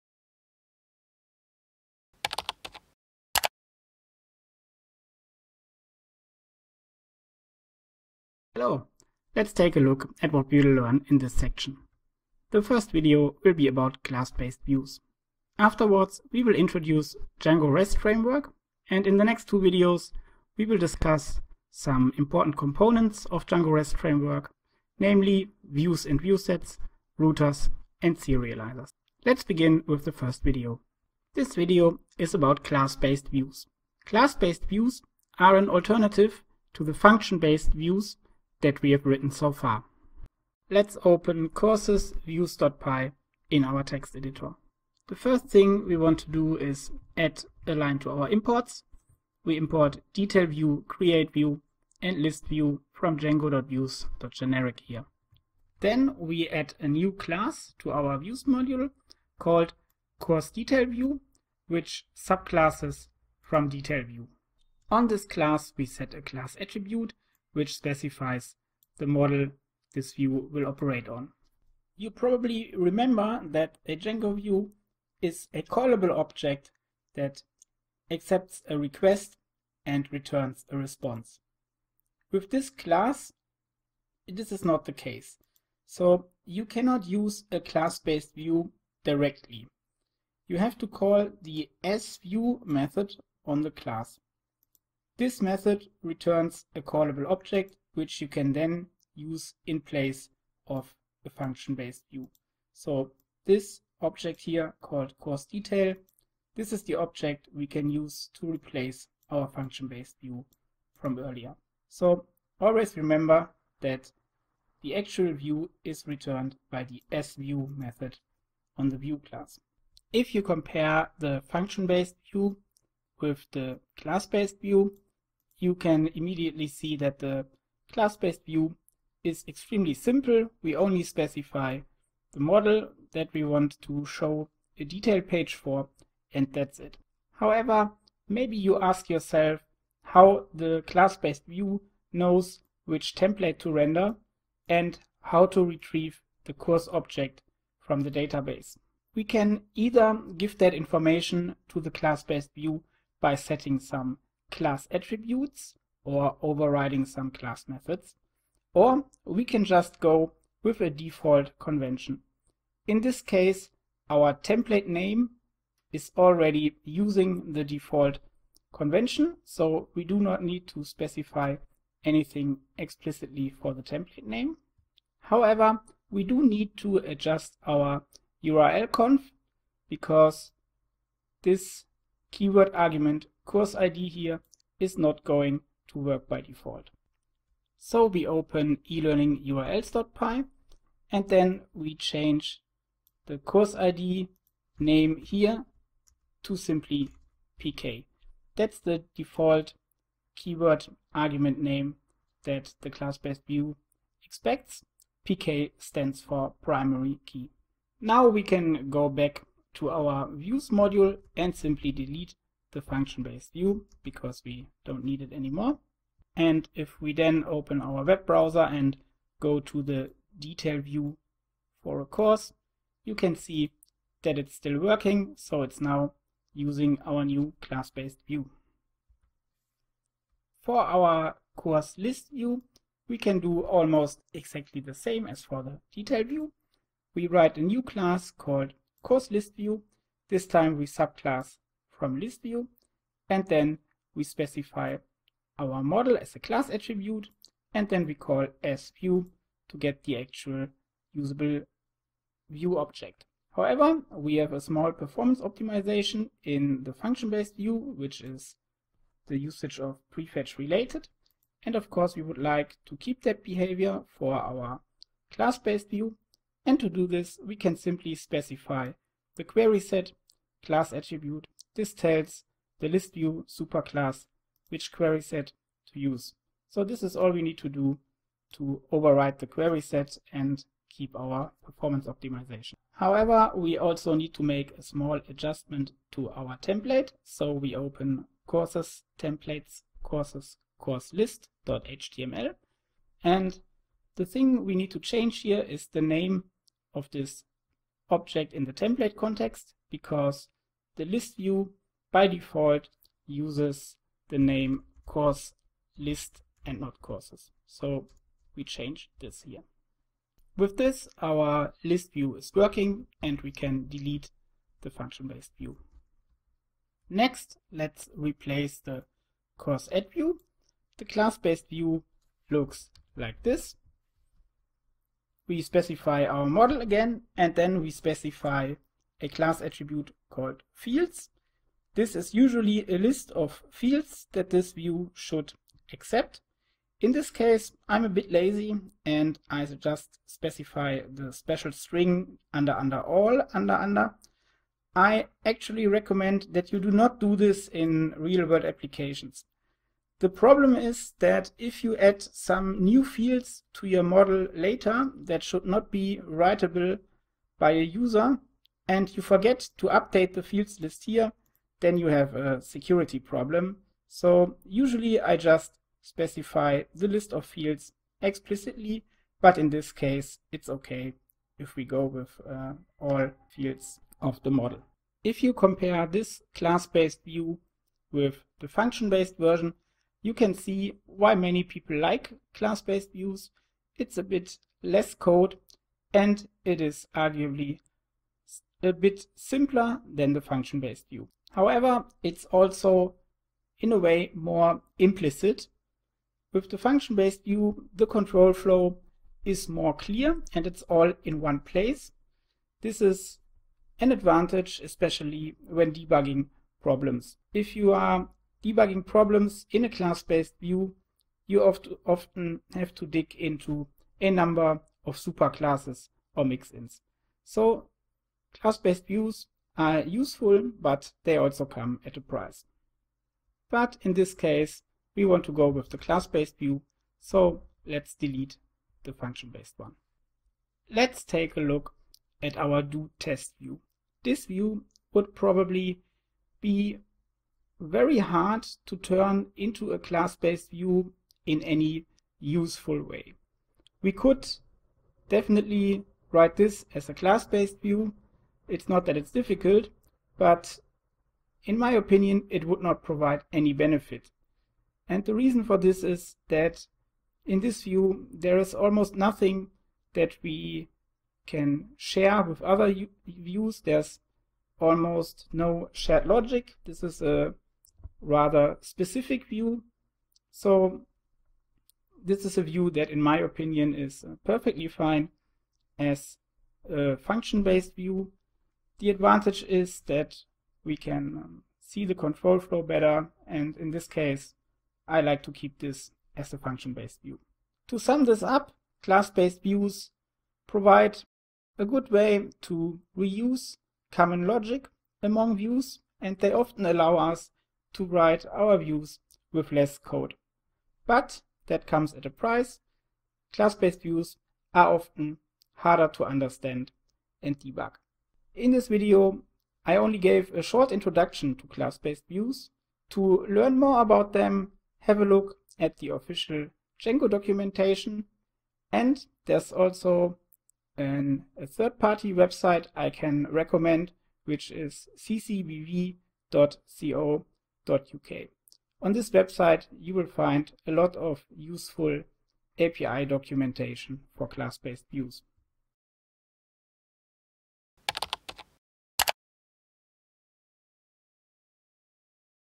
Django Projects: E-Learning Portal : Class-based Views | packtpub.com
The aim of this video is learn about class based views and how to use them in Django.
• Understand what class-based views are
• Use the class-based views in Django
• Reuse the common functionalities between various views